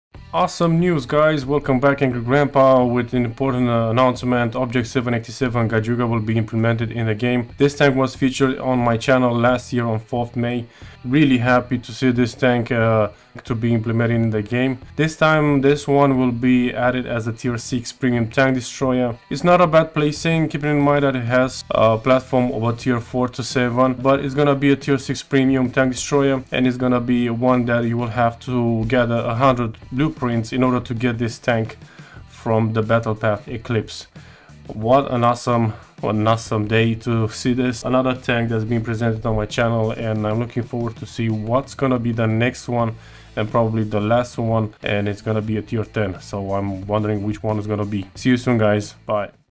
Obj. 787 ''Gadyuka''/ Viper Tier 6 TD in the upcoming Eclipse Battle Path
First was 2S38 DERIVATSIYA-PVO & ADDED as tier 8 AFV.
Second was Lynx & ADDED as tier 10 AFV.
Third is OTOMATIC & WILL BE ADDED as tier 9 TD.
Fourth is Flakpanzer Gepard 1 & WILL BE ADDED AS TIER 8 AFV.
Fifth is Obj. 787 Gadyuka/ Viper & WILL BE ADDED AS A TIER 6 TD!!!